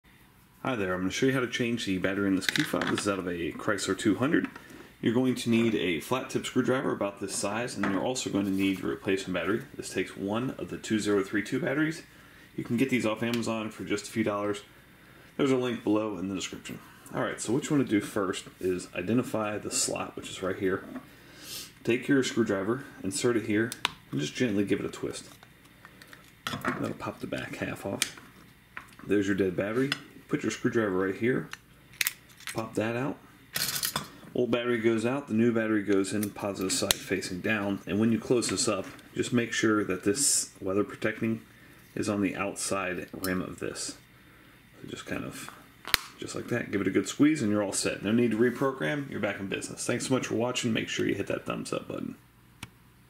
Chrysler 200 / 300 Key Fob Battery Replacement - EASY DIY
2032 Battery you need
Here is a quick and easy demonstration on how to change the battery in your Chrysler key fob. This specific one is out of a Chrysler 200. All you need is your new coin cell battery and a flat tip screwdriver. Use the screwdriver to pry the small slot that is near the base of the metal shaft. Carefully remove the weak / dead battery and put your new 2032 battery in with the + side facing down. Snap the two halves back together making sure the seal isn't pinched. You're all set!

Chrysler / Dodge 4-Button Remote Head Key (FCC ID: OHT692427AA) 315Mhz -Kee-Co
Part Number: KB-CHRY-04-4-AA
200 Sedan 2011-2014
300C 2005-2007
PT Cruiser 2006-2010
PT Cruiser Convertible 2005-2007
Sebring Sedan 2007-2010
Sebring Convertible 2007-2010